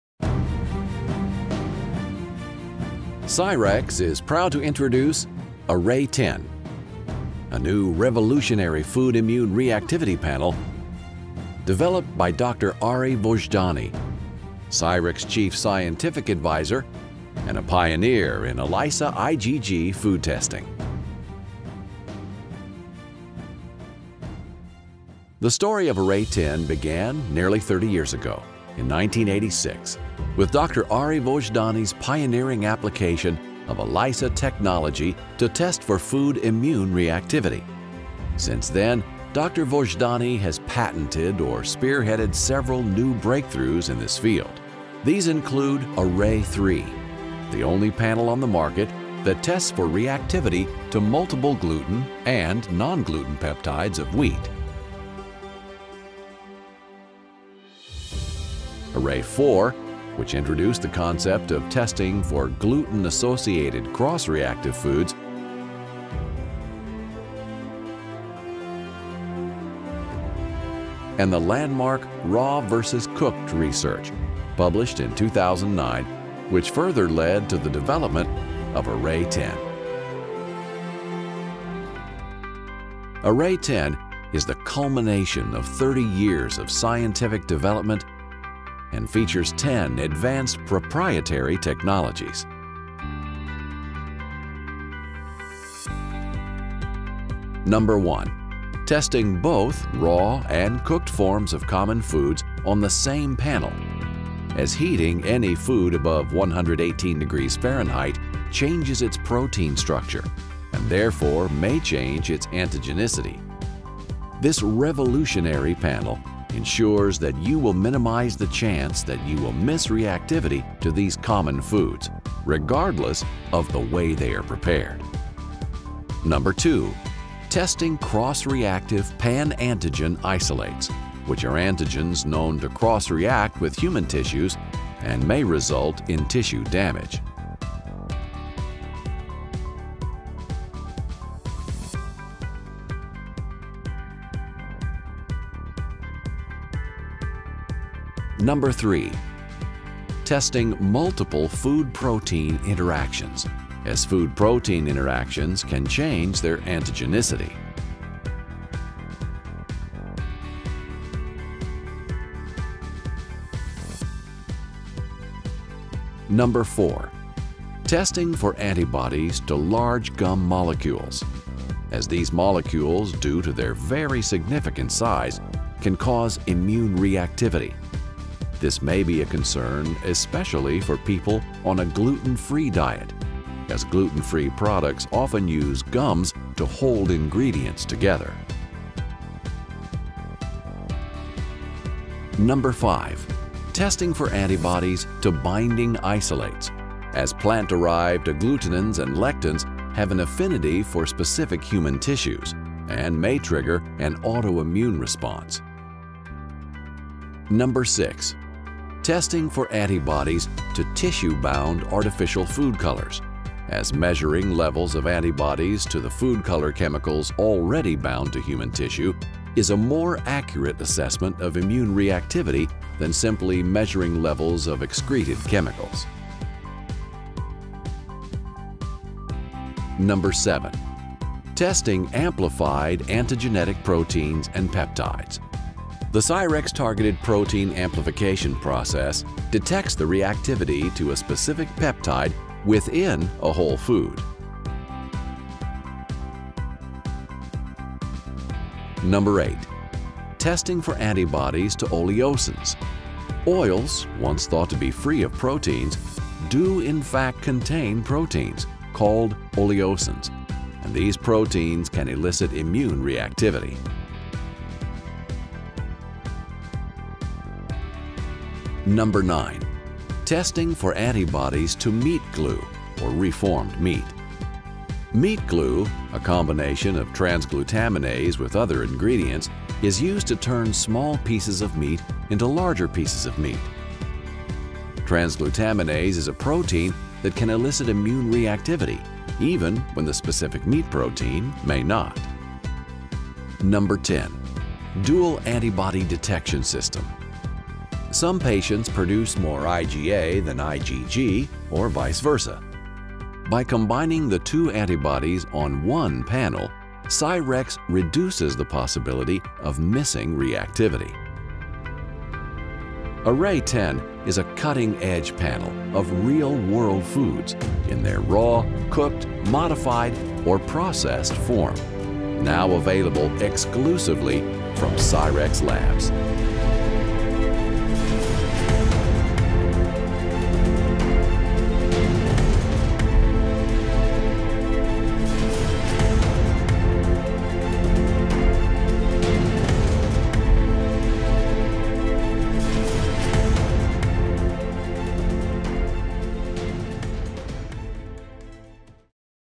Array 10 — Multiple Food Immune Reactivity Screen
Array 10 — Multiple Food Immune Reactivity Screen™
This unique, revolutionary panel measures reactivity to 180 food antigens in the cooked, raw, modified or processed form on the same panel. Assists in early detection of dietary-related triggers of autoimmune reactivity and monitors the effectiveness of customized dietary protocols.